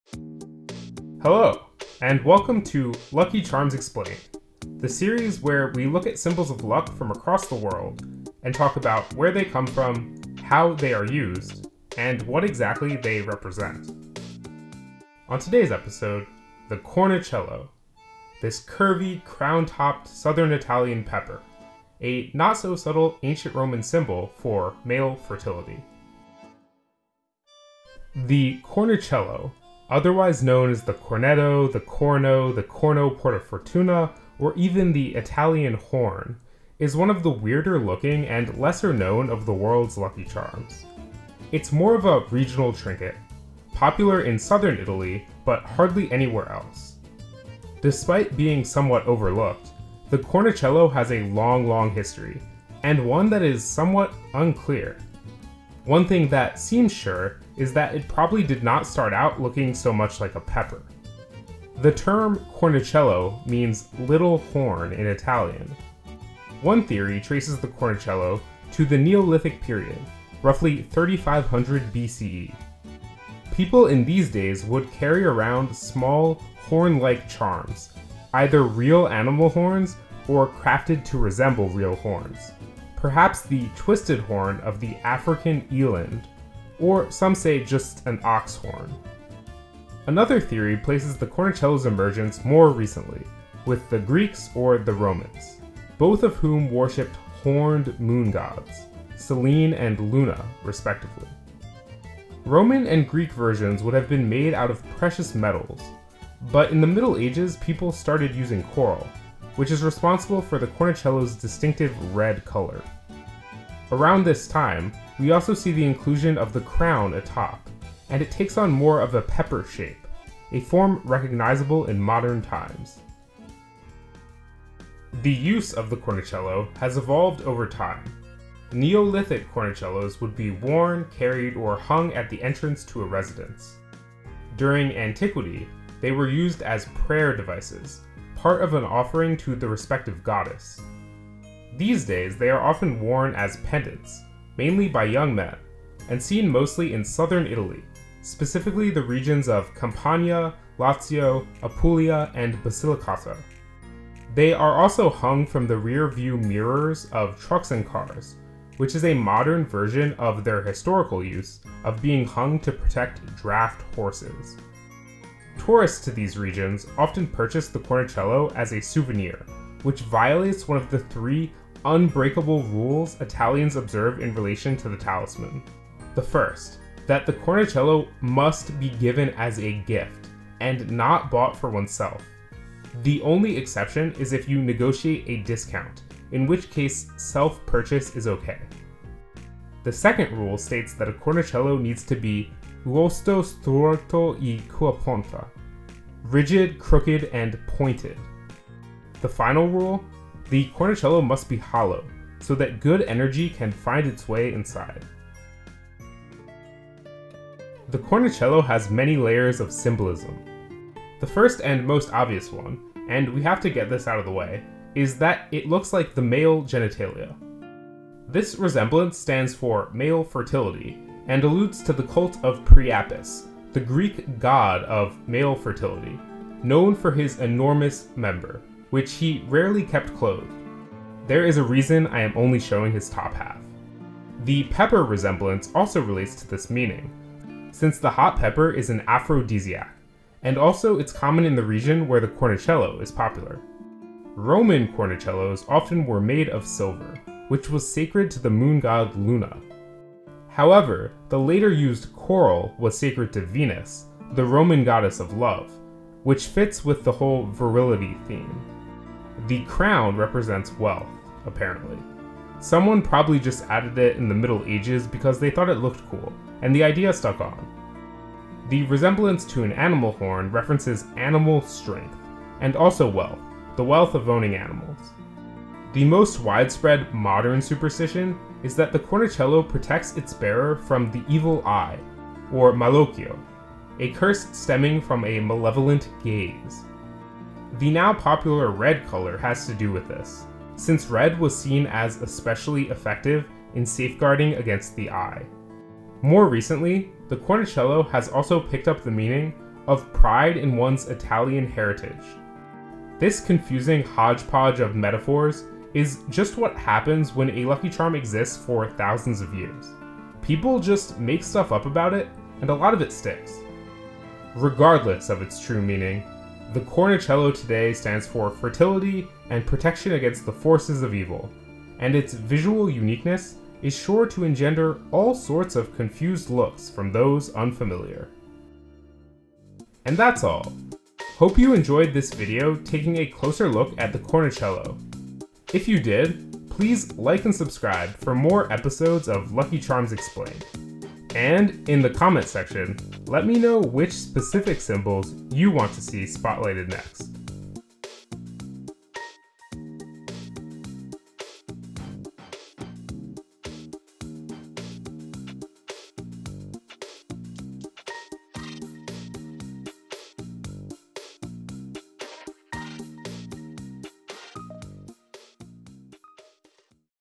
Cornicello (Italian Horn) - Lucky Charms Explained
Is it a pepper? Is it a phallus? Something else? Find out. The origins and deeply layered symbolism behind one of Italy's weirdest and most confusing talismans of good luck.

00:00 INTRO
00:27 ORIGIN
02:05 USAGE
03:27 MEANING
05:30 OUTRO